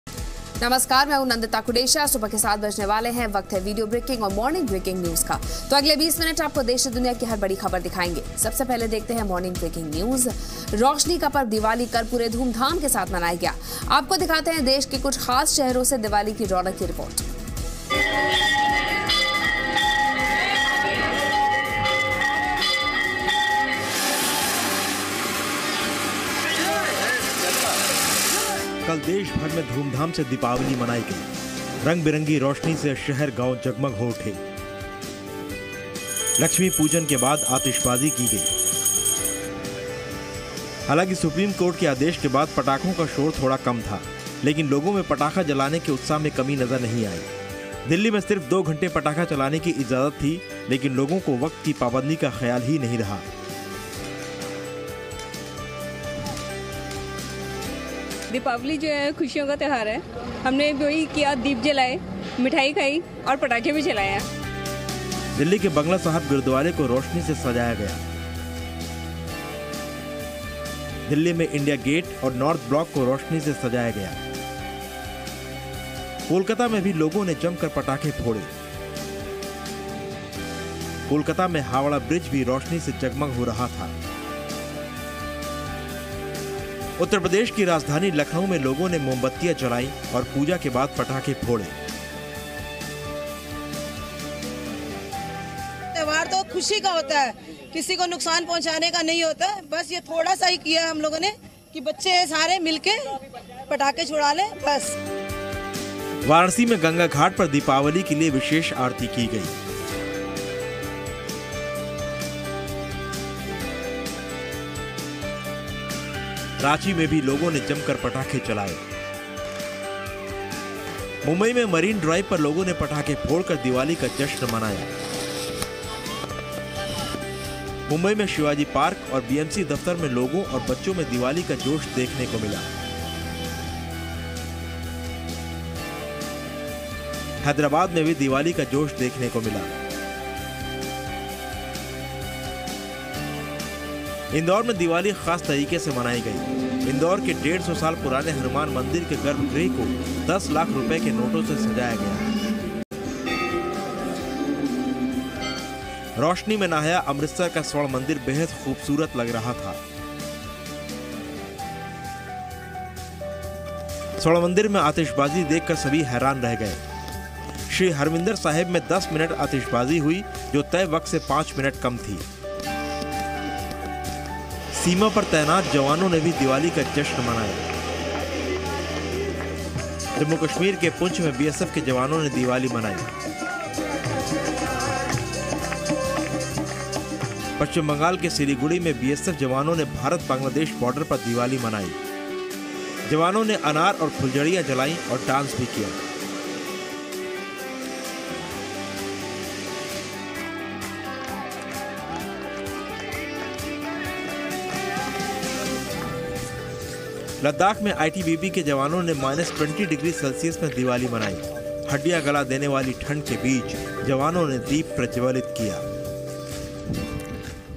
Morning Breaking: India celebrates festival of lights, Diwali with zeal
India celebrated Diwali on Wednesday with great zeal. Watch this video to know the latest news.